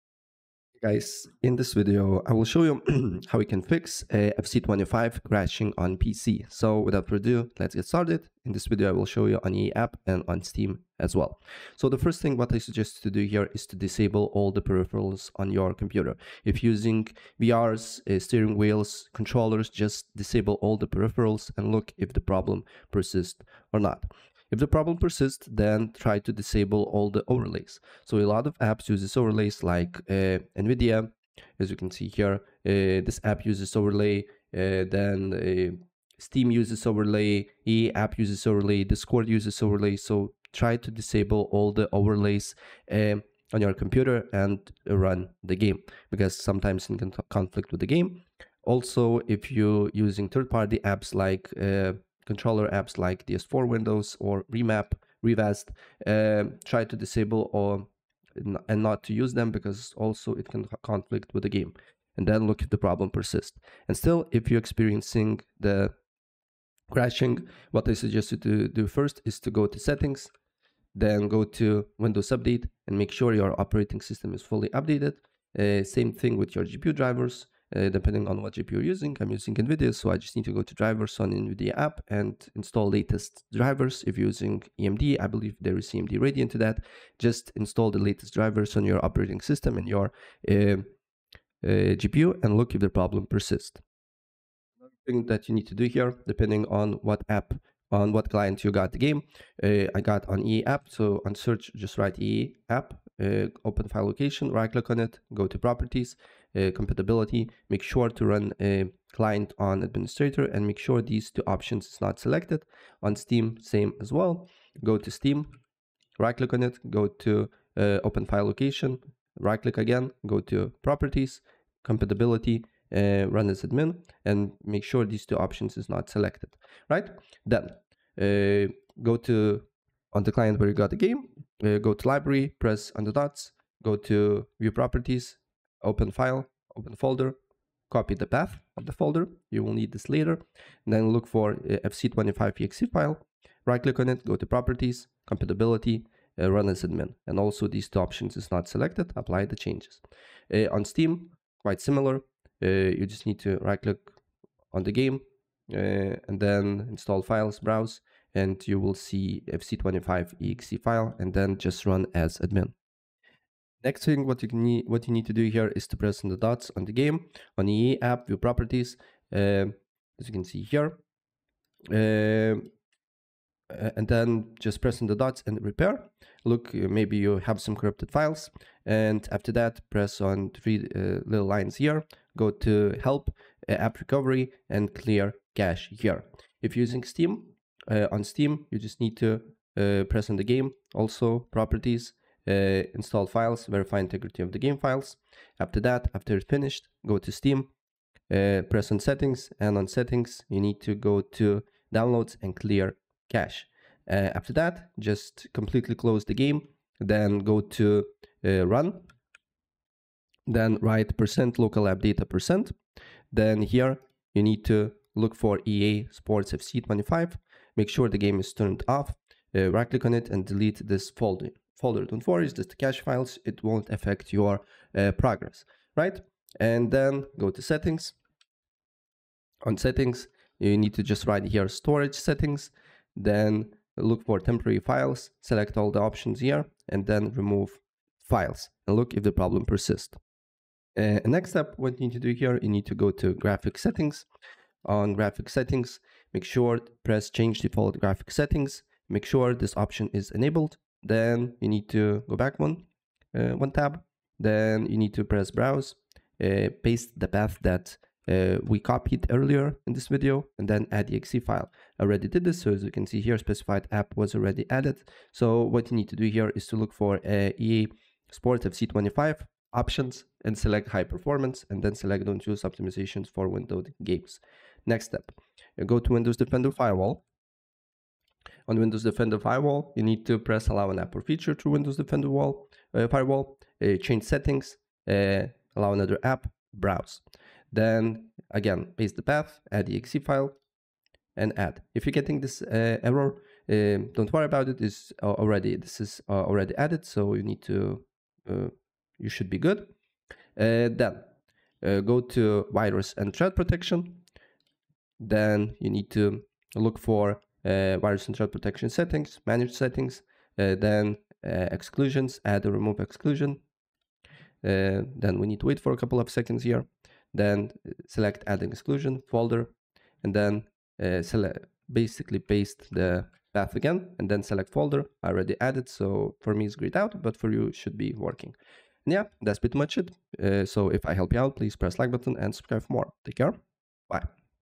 How To Fix FC 25 Crashing & Black Screen on PC - Full Guide
In this video I will show you How To Fix FC 25 Crashing & Black Screen on PC It's really easy and it will take you less than a minute to do it!